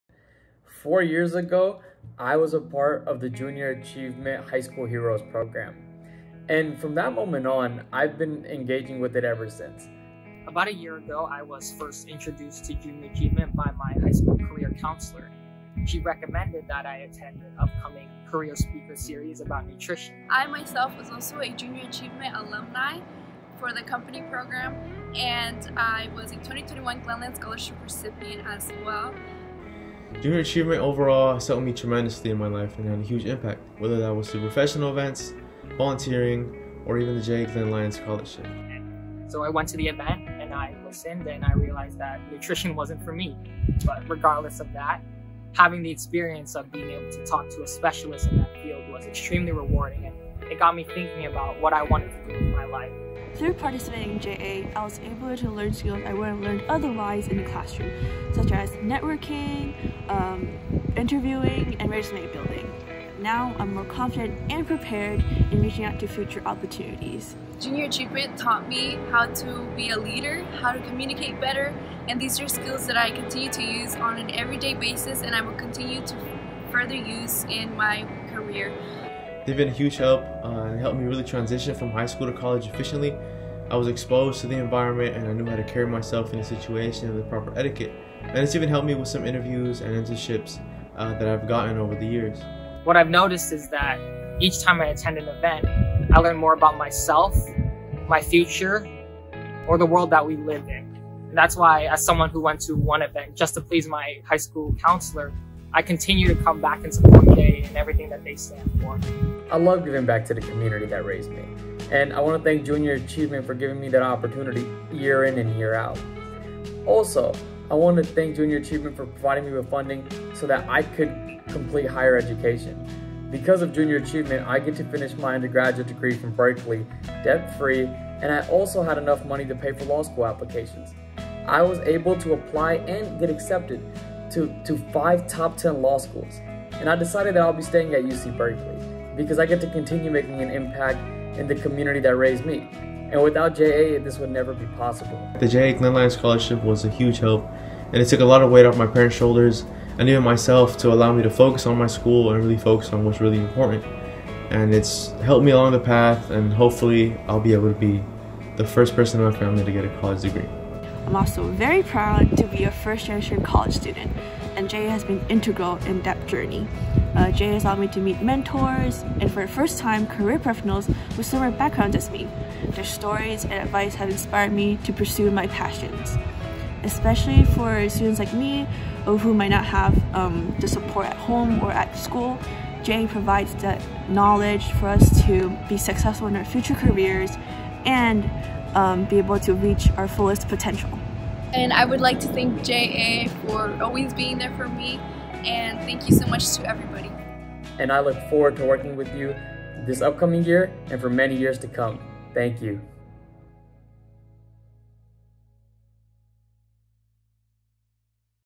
Why JA?
Hear from just some of our students why Junior Achievement matters. Proceeds will benefit our financial literacy, entrepreneurship, and workforce readiness programs, improving the lives of 50,000 K-12 students with the skills necessary to be successful in college and career.